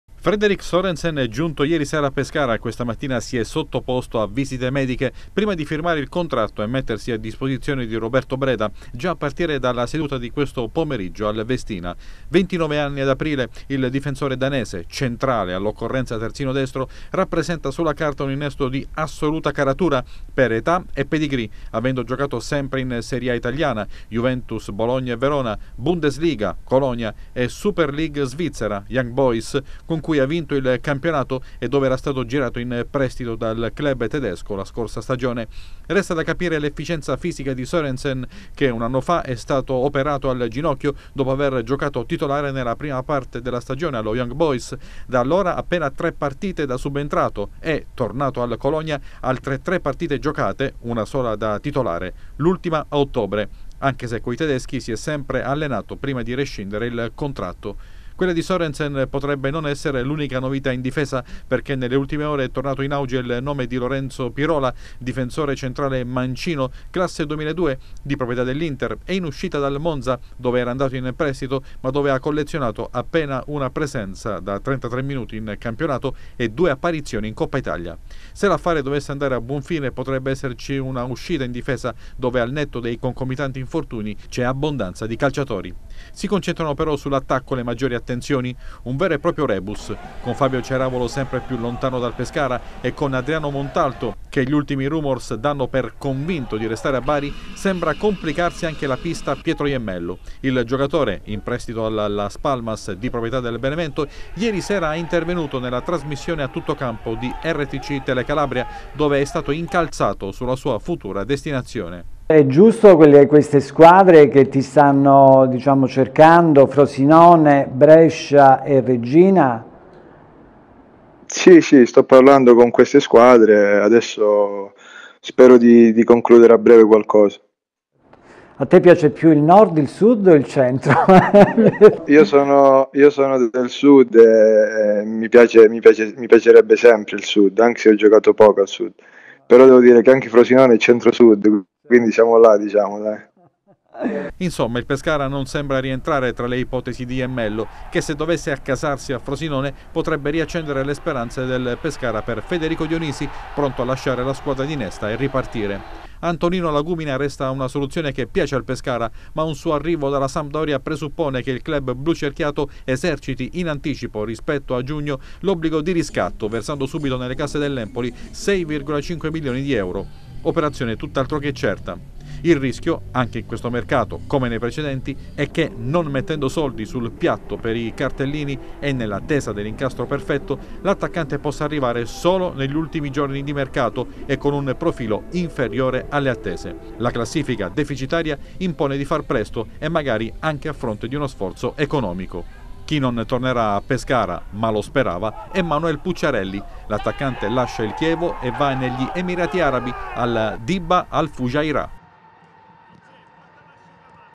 Pescara, ecco Sorensen. Iemmello… si smarca
Visite mediche stamani per il nuovo difensore del Pescara Frederik Sorensen. I biancazzurri sono vicini anche Lorenzo Pirola (ora al Monza ma di proprietà dell’Inter). Sul fronte attaccante c’è ancora tanta incertezza. Ieri sera Iemmello, intervenuto ad una emittente calabrese, non ha citato Pescara come sua possibile destinazione